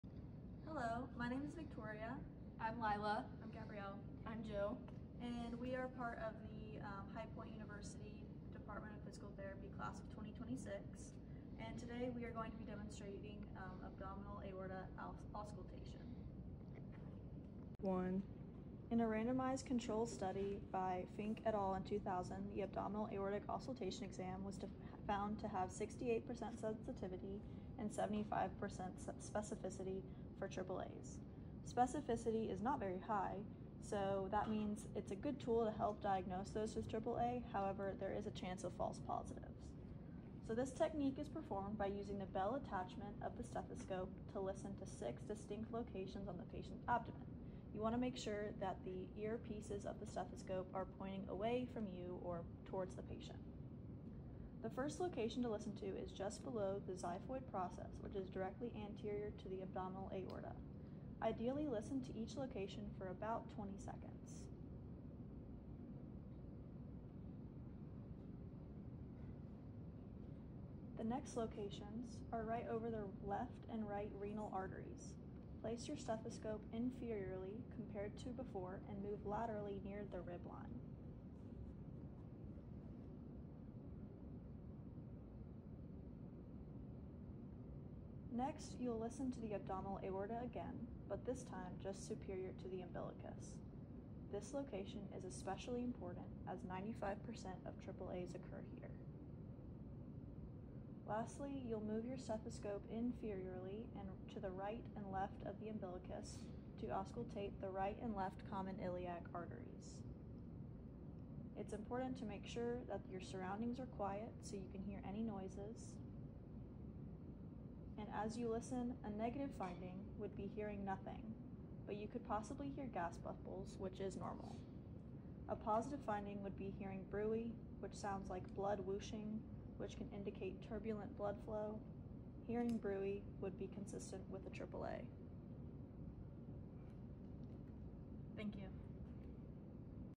Abdominal Aortic Auscultation
High Point University DPT 2nd year students showing abdominal aortic auscultation